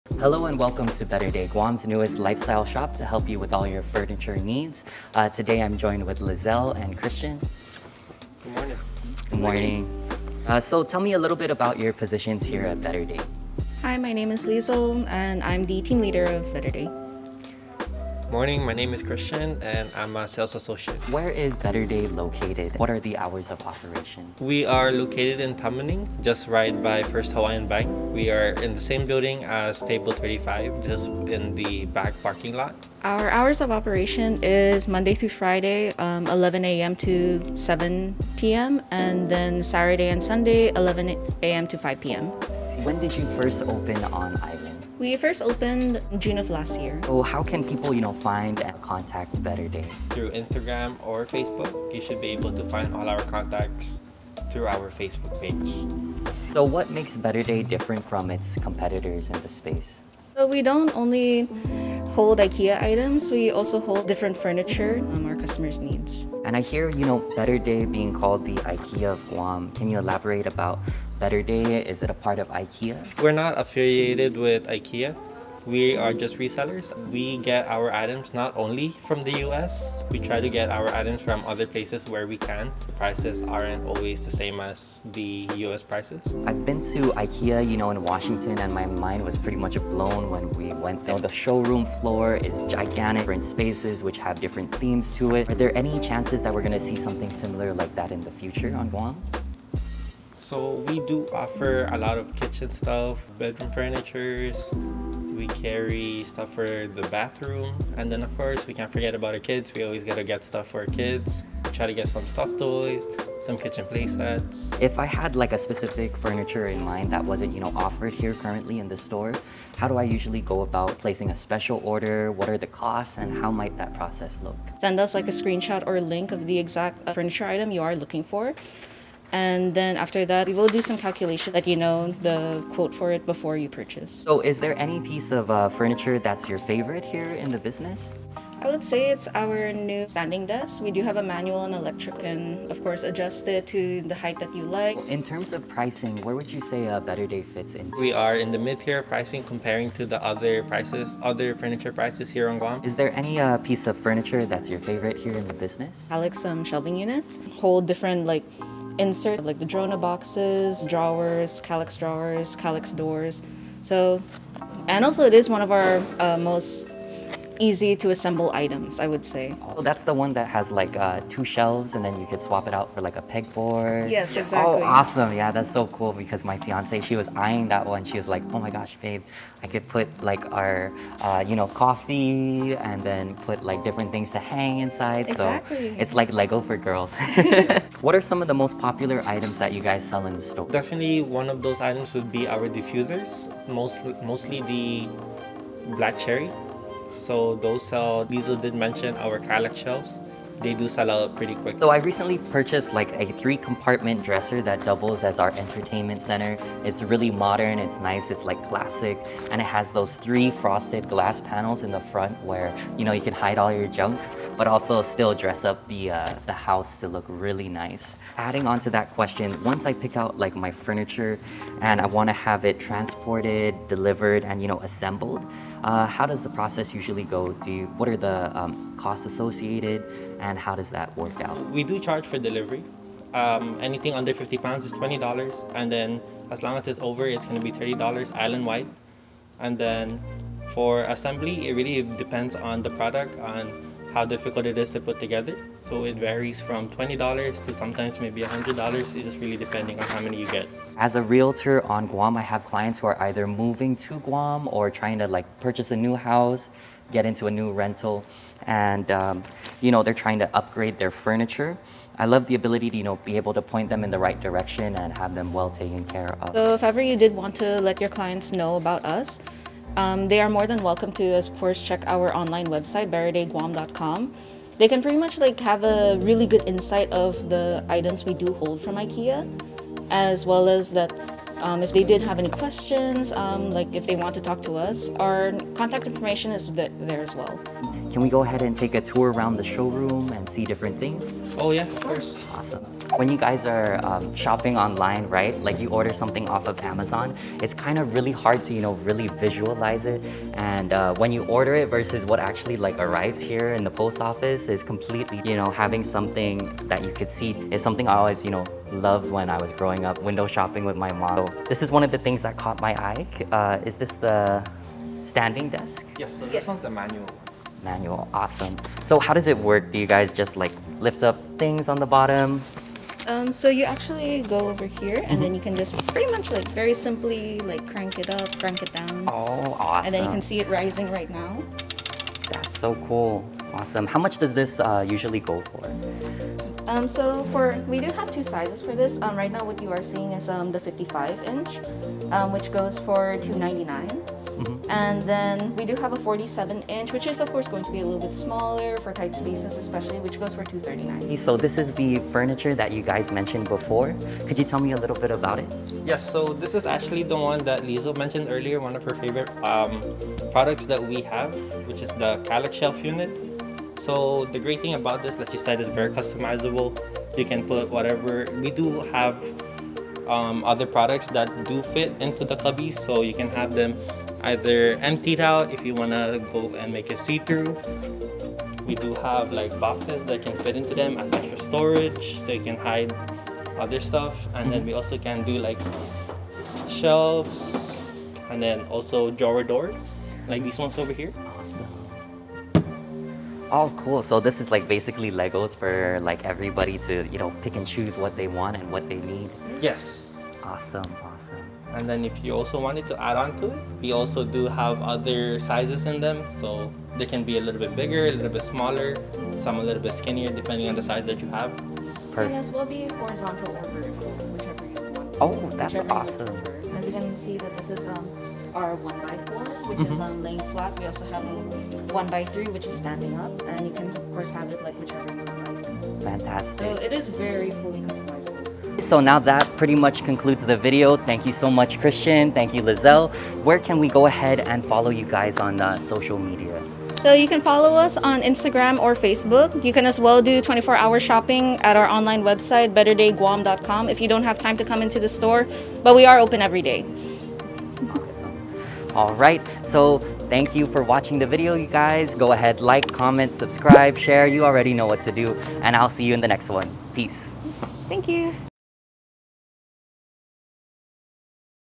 Guam's Newest Lifestyle Shop | Better day Interview & Store tour!
Today I tour & interview Betterday Guam's newest Lifestyle & furniture shop!

Interview w/Leezle (Team Leader) & Christian (Sales Associate) at Betterday.

What does Betterday offer, what are the hours of operation, and what might I see at the store?

Stay and watch the video to find out more information.

                        Location: 655 S Marine Corp Drive Tamuning, GU 96913